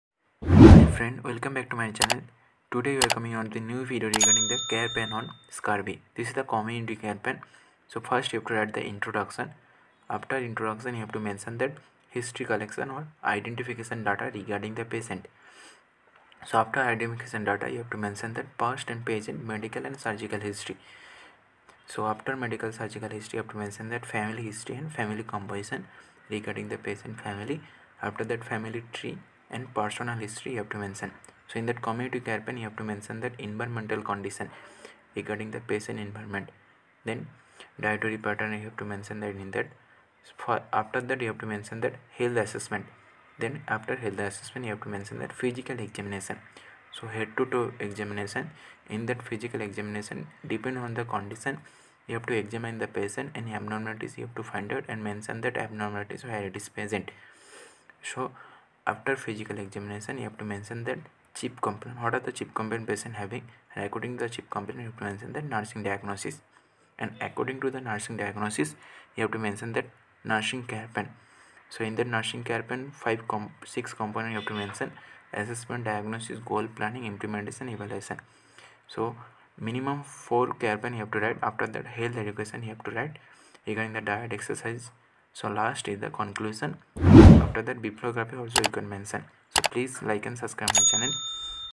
care plan on scurvy | scurvy care plan in nursing | scurvy ncp
in this video you can learn about the how to write the care plan on scurvy for the child or adult also nursing care plan. process of writing care plan in nursing. how to give care to the scurvy patient . the process of writing care plan
🟢Your Queries --